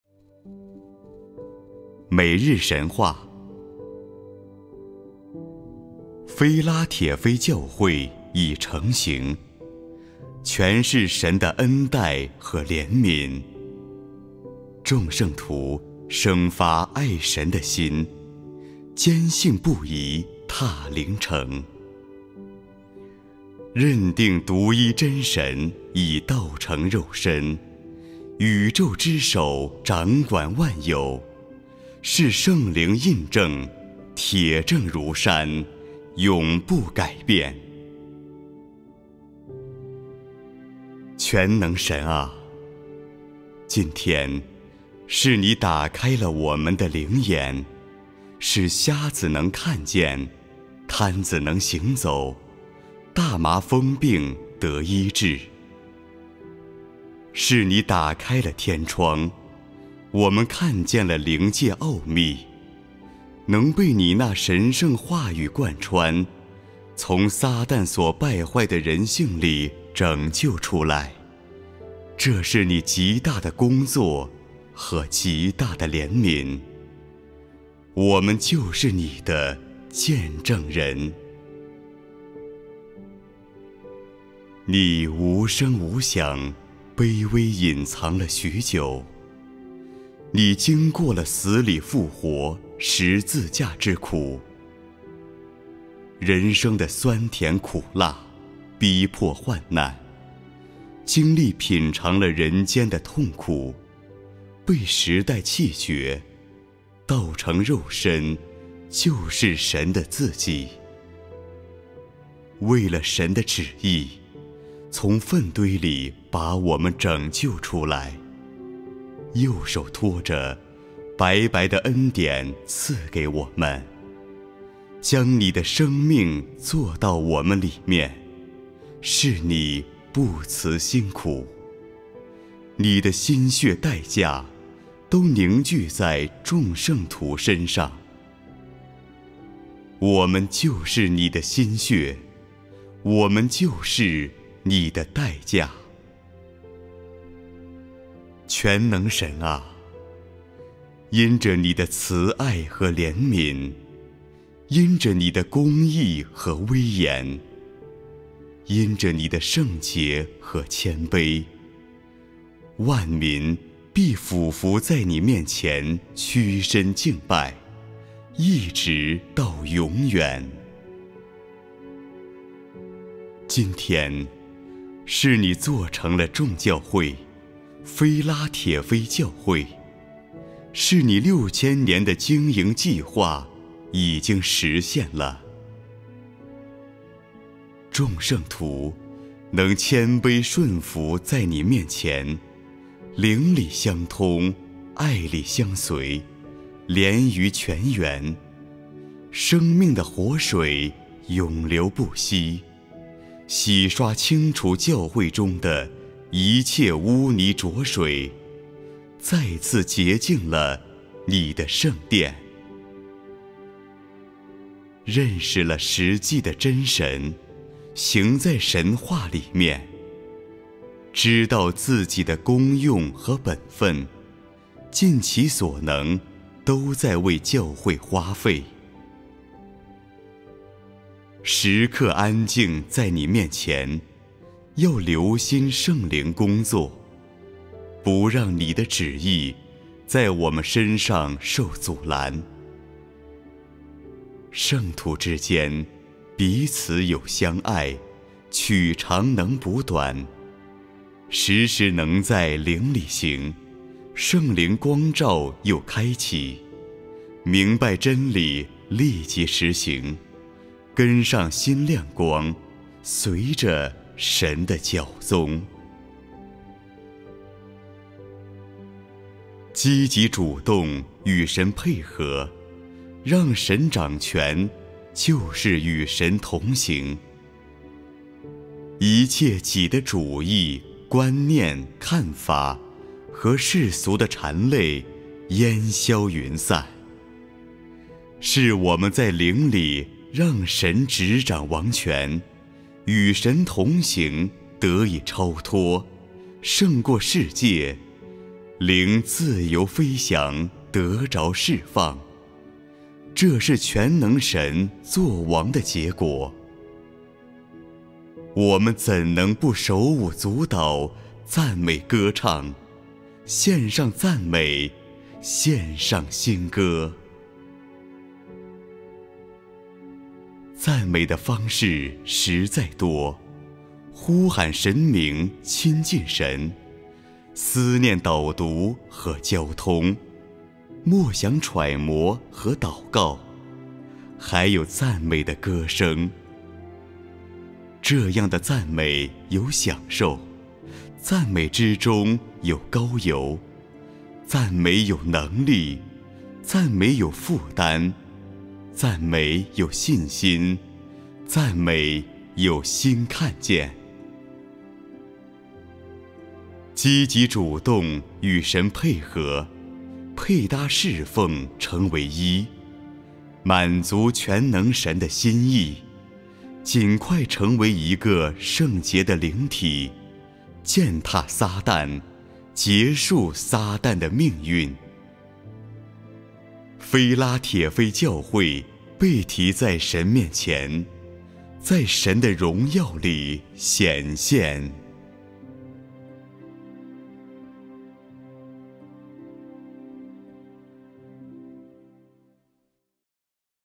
每日神話 - 神顯現作工系列 選段47
非拉鐵非教會已成形，全是神的恩待和憐憫，衆聖徒生發愛神的心，堅信不移踏靈程。認定獨一真神已道成肉身，宇宙之首掌管萬有，是聖靈印證，鐵證如山！永不改變！
　　全能神啊！今天是你打開了我們的靈眼，使瞎子能看見，癱子能行走，大痲瘋病得醫治；是你打開了天窗，我們看見了靈界奥秘，能被你那神聖話語貫穿，從撒但所敗壞的人性裏拯救出來，這是你極大的工作和極大的憐憫。我們就是你的見證人！
　　你無聲無響卑微隱藏了許久，你經過了死裏復活，十字架之苦，人生的酸甜苦辣，逼迫患難，經歷品嘗了人間的痛苦，被時代弃絶，道成肉身就是神的自己。為了神的旨意，從糞堆裏把我們拯救出來，右手托着，白白的恩典賜給我們。將你的生命作到我們裏面，是你不辭辛苦，你的心血代價都凝聚在衆聖徒身上。我們就是你的心血，我們就是你的代價。
　　全能神啊！因着你的慈愛和憐憫，因着你的公義和威嚴，因着你的聖潔和謙卑，萬民必俯伏在你面前屈身敬拜，一直到永遠。
　　今天是你作成了衆教會——非拉鐵非教會，是你六千年的經營計劃已經實現了。衆聖徒能謙卑順服在你面前，靈裏相通愛裏相隨，聯于泉源，生命的活水涌流不息，洗刷清除教會中的一切污泥濁水，再次潔净了你的聖殿。認識了實際的真神，行在神話裏面，知道自己的功用和本分，盡其所能都在為教會花費。時刻安静在你面前，要留心聖靈工作，不讓你的旨意在我們身上受阻攔。聖徒之間彼此有相愛，取長能補短。時時能在靈裏行，聖靈光照又開啓，明白真理立即實行，跟上新亮光，隨着神的脚踪。
　　積極主動與神配合，讓神掌權就是與神同行。一切己的主意、觀念、看法和世俗的纏累烟消雲散。是我們在靈裏讓神執掌王權，與神同行得以超脱，勝過世界，靈自由飛翔得着釋放，這是全能神作王的結果。我們怎能不手舞足蹈贊美歌唱，獻上贊美，獻上新歌。
　　贊美的方式實在多：呼喊神名，親近神，思念禱讀和交通，默想揣摩和禱告，還有贊美的歌聲。這樣的贊美有享受，贊美之中有膏油，贊美有能力，贊美有負擔，贊美有信心，贊美有新看見。
　　積極主動與神配合，配搭事奉成為一，滿足全能神的心意，盡快成為一個聖潔的靈體，踐踏撒但，結束撒但的命運。非拉鐵非教會被提在神面前，在神的榮耀裏顯現。
                                                                     ——《話・卷一　神的顯現與作工・基督起初的發表・第二篇》